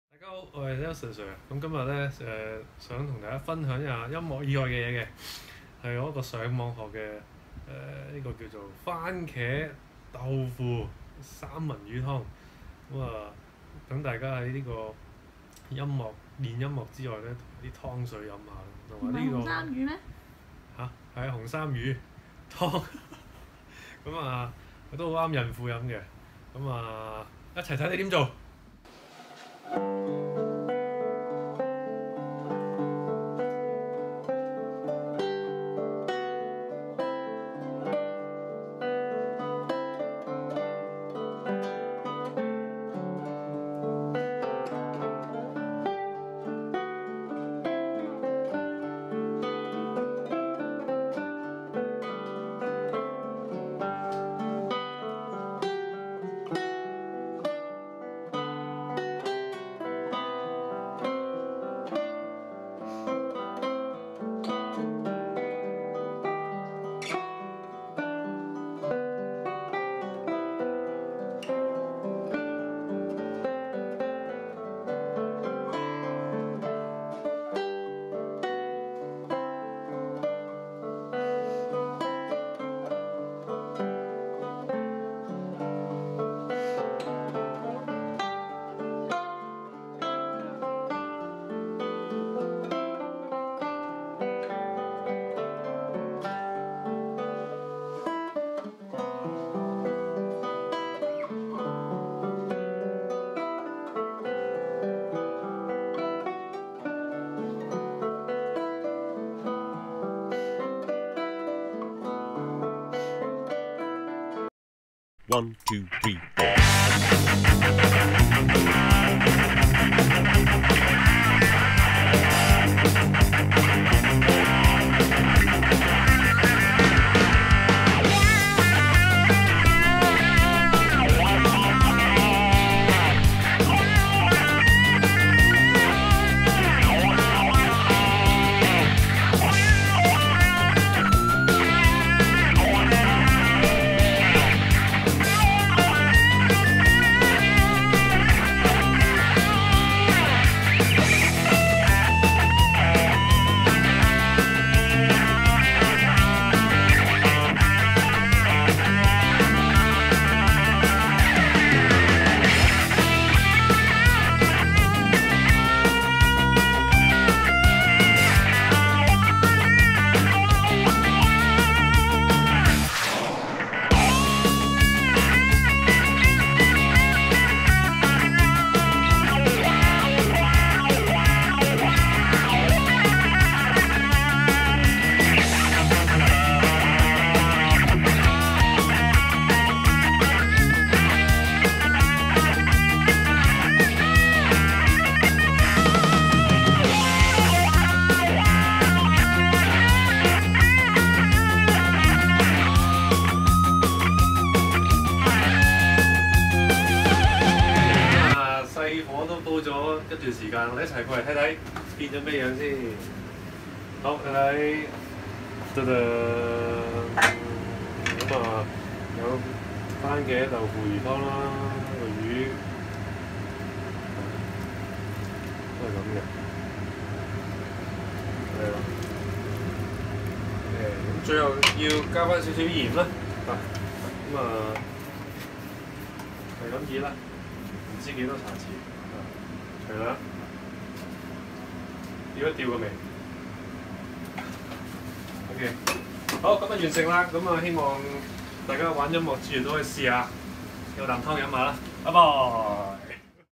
蕃茄紅衫魚豆腐湯
上網睇完再做一次俾大家睇，同大家分享嘅其實最主要都係「音樂」。
當然真係有啖湯飲下係好D 嘅。haha

希望大家中意。